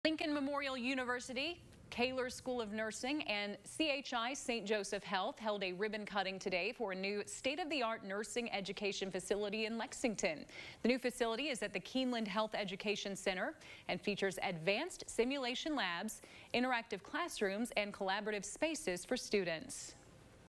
New nursing facility ribbon cutting
Lincoln Memorial University, Caylor School of Nursing and CHI Saint Joseph Health held a ribbon cutting event on Wednesday for a new state-of-the-art nursing education facility in Lexington. The new facility is at the Keeneland Health Education Center and features advanced labs, interactive classrooms and new spaces for students.